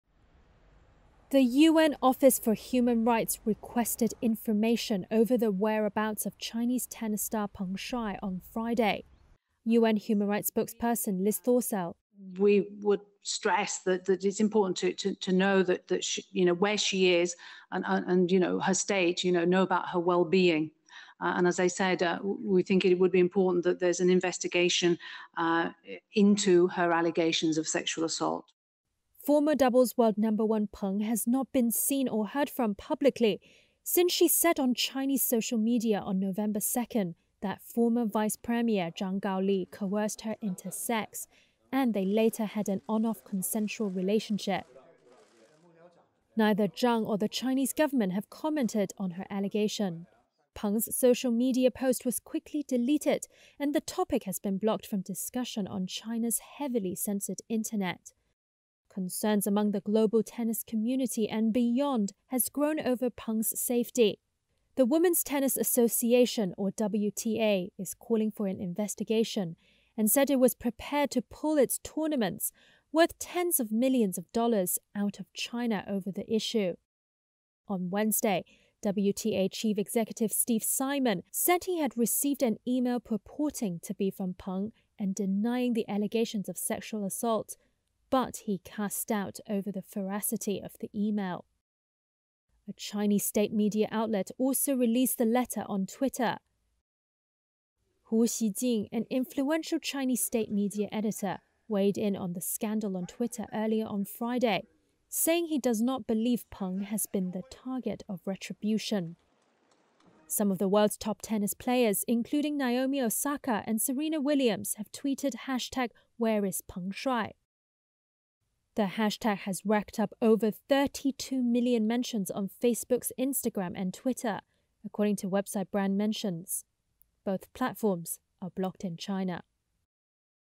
UN asks for info on missing Chinese tennis star Peng Shuai
The UN office for human rights requested on Friday information regarding Chinese tennis star Peng Shuai's whereabouts and calls for an investigation into her allegations of sexual assault.

VIDEO: REUTERS